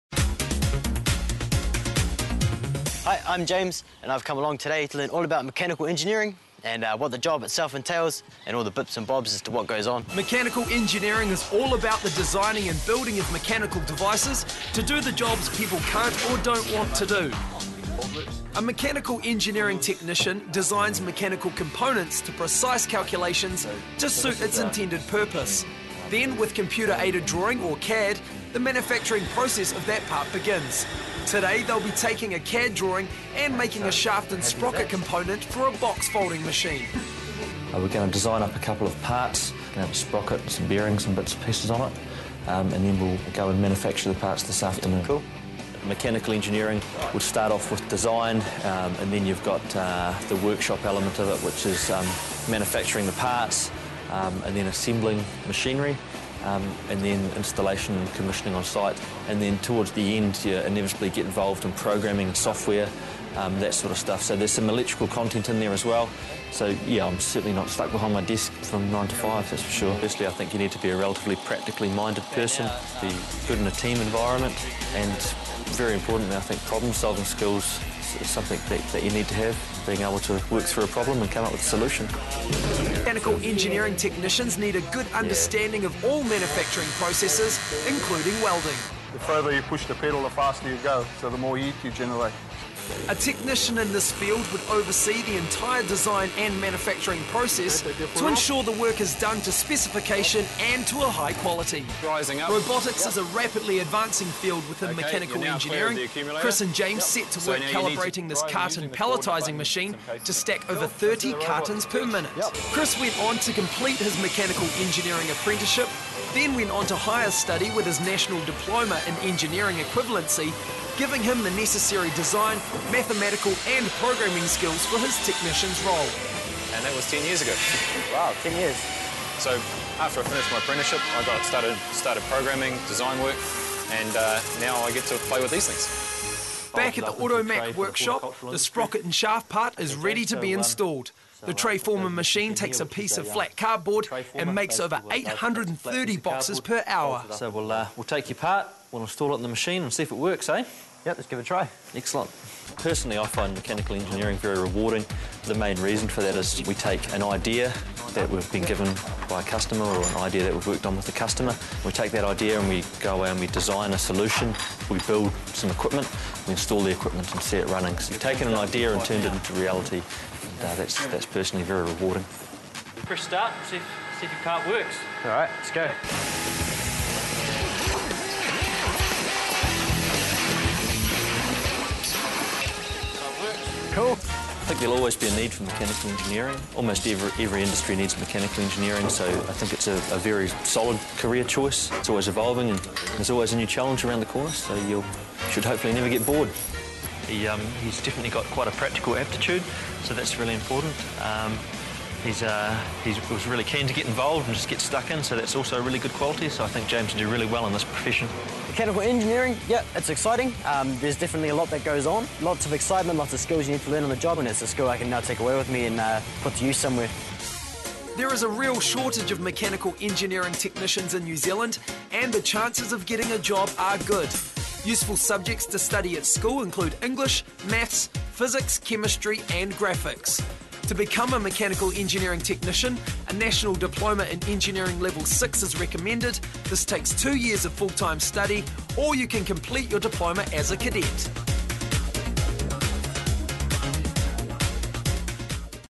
Just the Job: Mechanical Engineering
Introduction to an apprenticeship in the Mechanical Engineering industry.